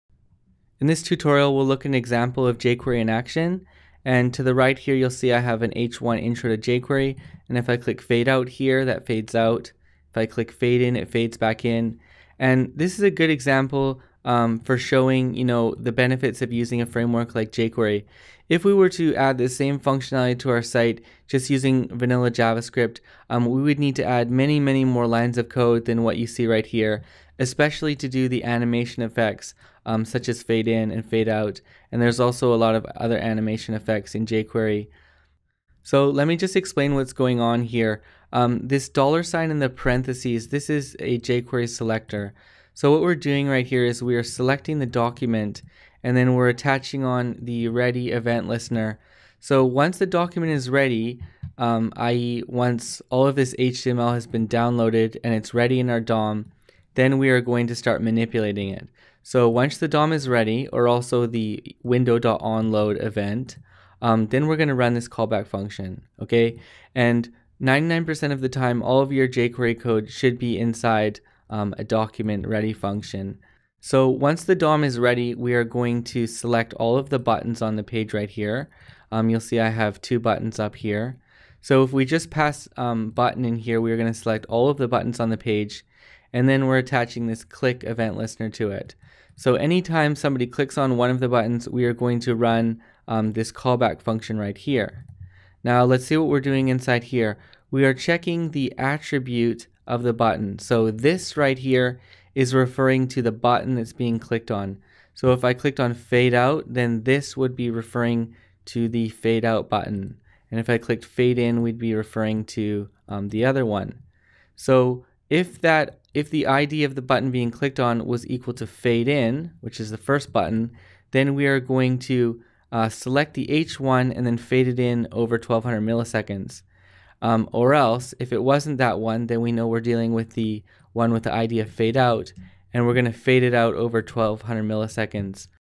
Beginner jQuery Tutorial 2 - Why place code within $(document).ready()
A simple jQuery example and why jQuery code is placed inside a $(document).ready() event handler.